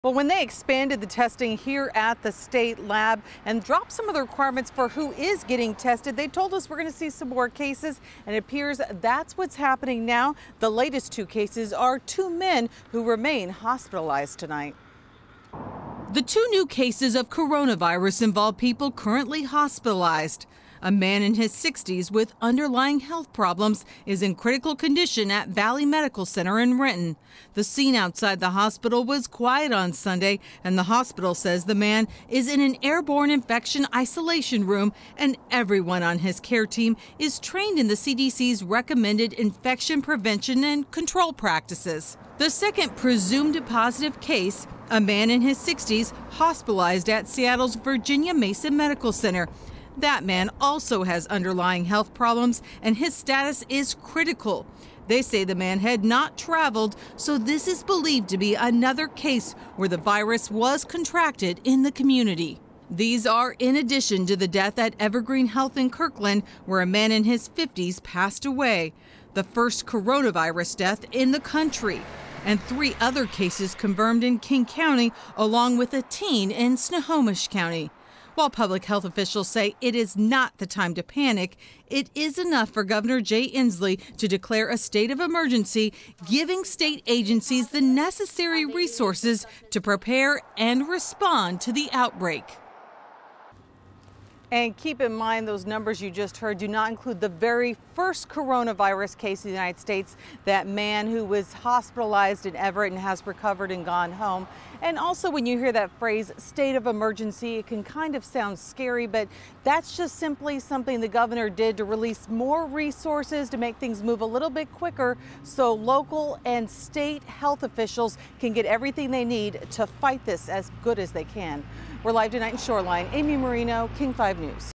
Two more people test positive for coronavirus in King County
Two men, who already had underlying health issues, tested positive for the coronavirus. So far, there are 8 confirmed cases of coronavirus in the state of Washington. Gov. Jay Inslee has issued a State of Emergency to get more resources for state health officials.